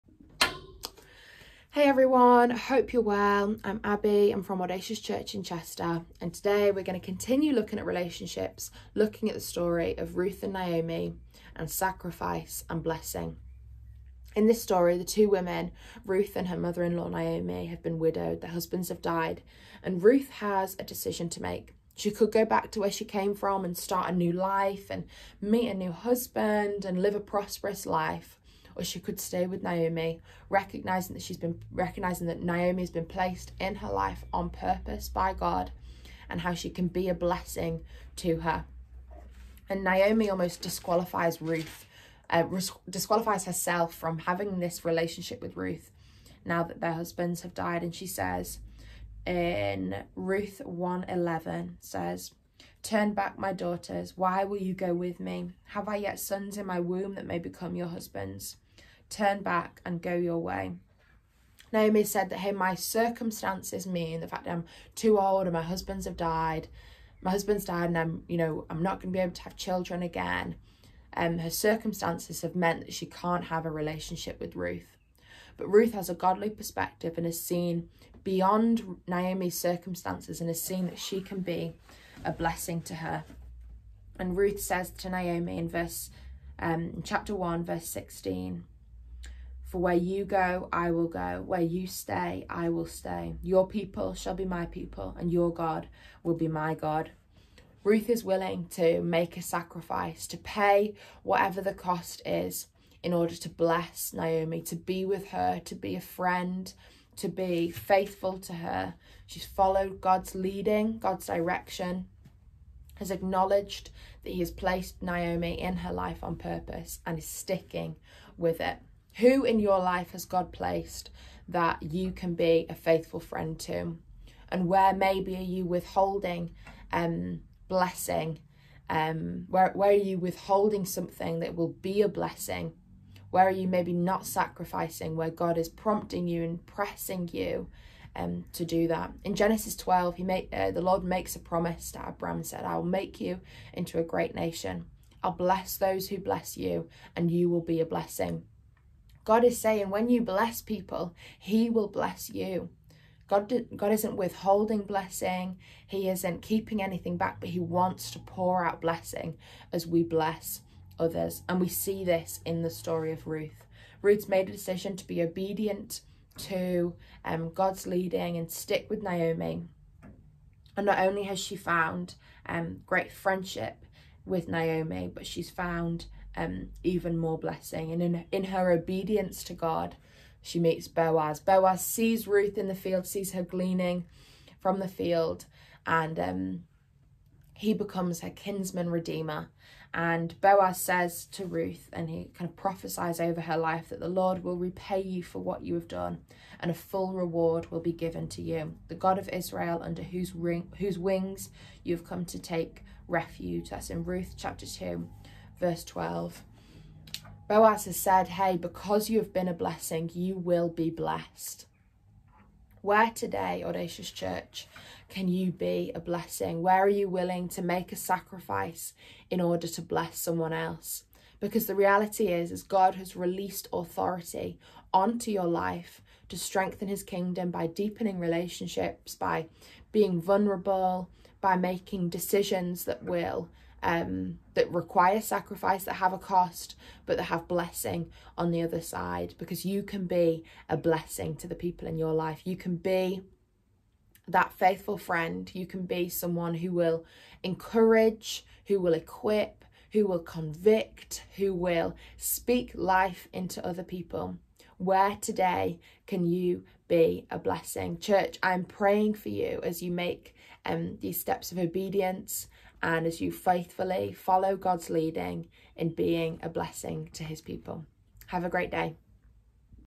!Audacious Daily Devotionals - Tuesday 7th June 2022
Today, Abby Watling part of our Chester Campus, continues our devotional series on relationships.